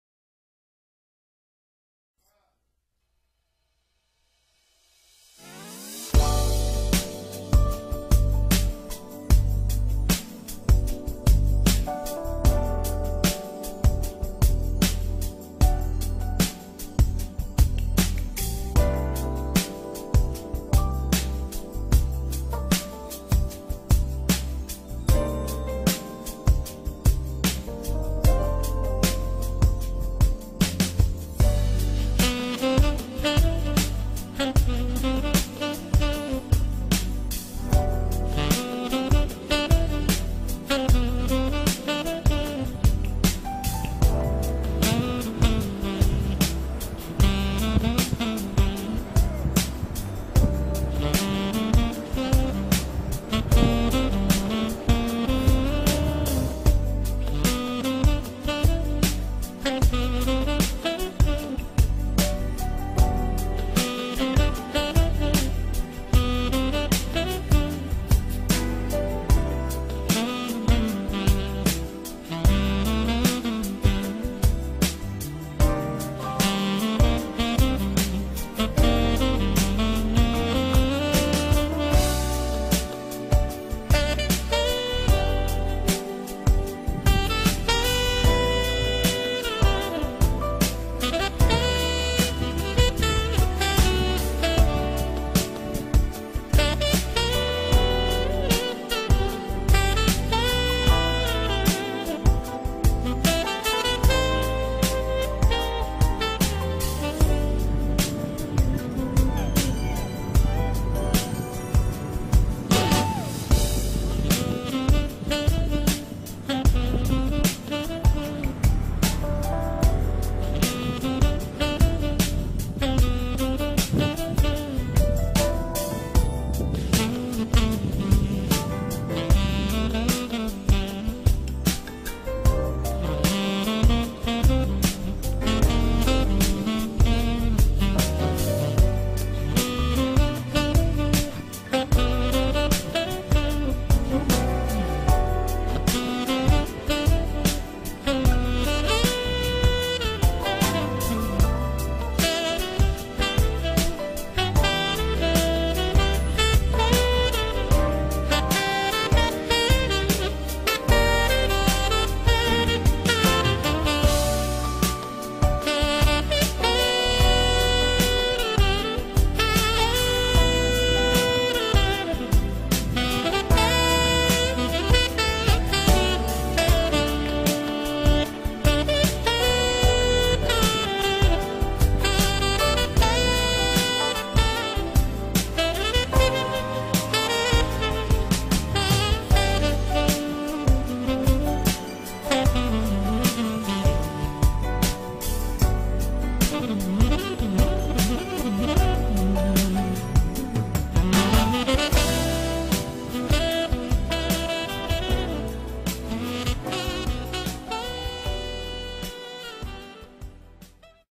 Redondo Beach Ca.,
October 4, 2011... fishing...background music by AMERICAN SMOOTH JAZZ  saxophonist Euge Groove..